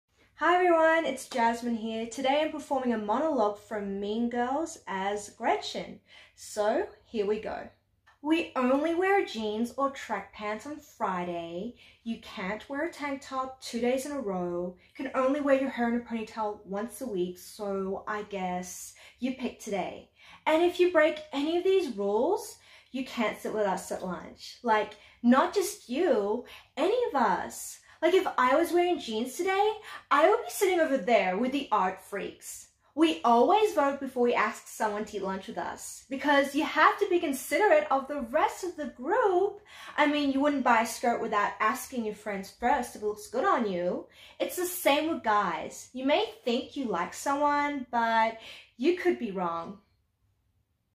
Self Tape - Gretchen - Mean Girls (2004)
Here is Monologue piece from a very popular movie

I love this piece personally because it gave me the ability to really become a DRAMATIC character and chase after a 'Scene Objective'

You are welcome to place your feedback in the comments ✨